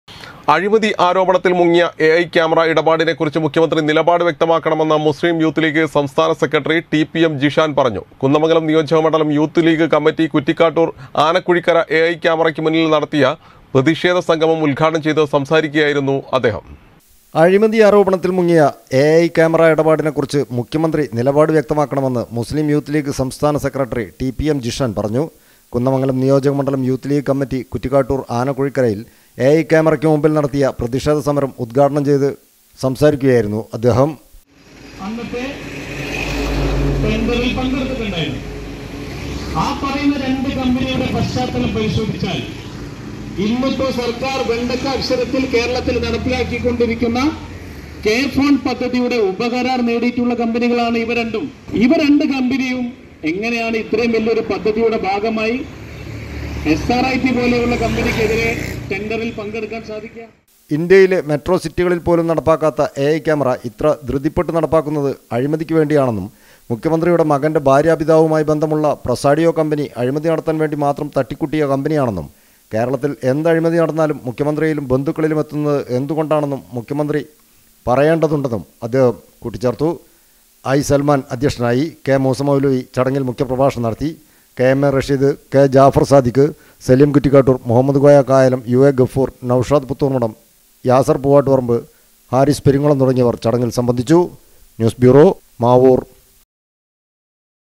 അഴിമതി ആരോപണത്തിൽ മുങ്ങിയ എ ഐ ക്യാമറ ഇടപാടിനെക്കുറിച്ച് മുഖ്യമന്ത്രി നിലപാട് വ്യക്തമാക്കണമെന്ന്
അഴിമതി ആരോപണത്തിൽ മുങ്ങിയ എ ഐ ക്യാമറ ഇടപാടിനെക്കുറിച്ച് മുഖ്യമന്ത്രി നിലപാട് വ്യക്തമാക്കണമെന്ന് മുസ്ലിം യൂത്ത് ലീഗ് സംസ്ഥാന സെക്രട്ടറി ടി.പി.എം ജിഷാൻ പറഞ്ഞു